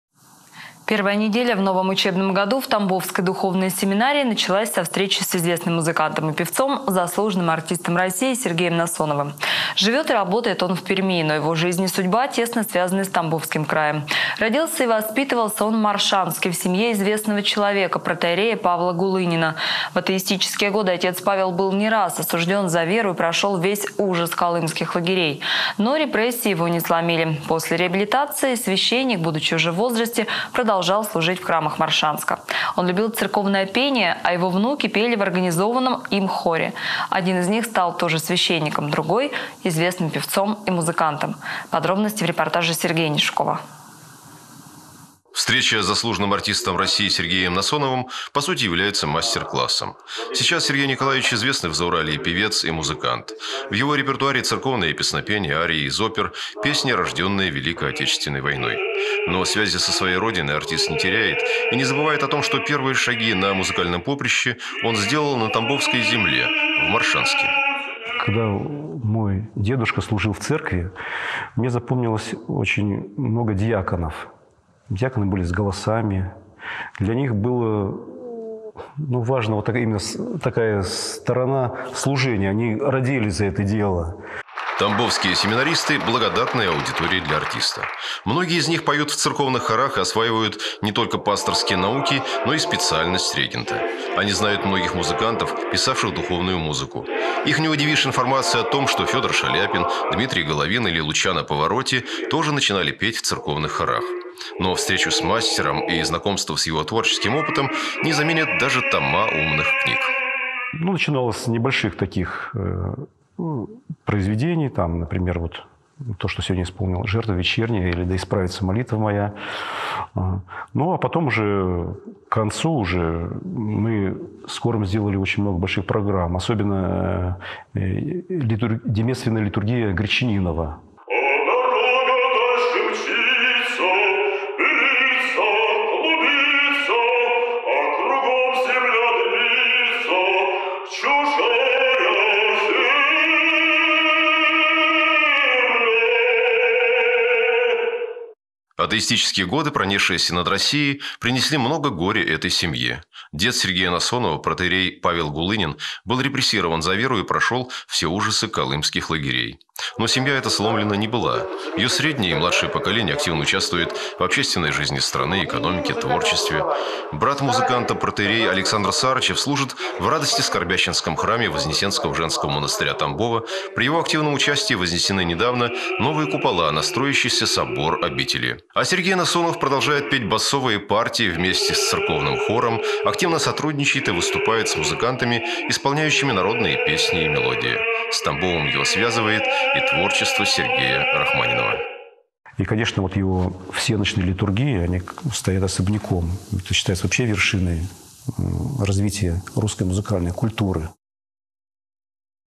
В новом учебном году в Тамбовской духовной сем
Первая неделя в новом учебном году в Тамбовской духовной семинарии началась со встречи с известным музыкантом и певцом - заслуженным артистом России Сергеем Насоновым. Живет и работает он в Перми, но его жизнь и судьба тесно связаны с тамбовским краем. Родился и воспитывался он в Моршанске в семье известного человека - протоиерея Павла Гулынина. В атеистические годы отец Павел был не раз осужден за веру и прошел весь ужас колымских лагерей. Но репрессии его не сломили. После реабилитации старый священник продолжал служить в храмах Моршанска. Он любил церковное пение, а его внуки пели в организованном им хоре. Один из них стал тоже священником. Другой - известным певцом и музыкантом.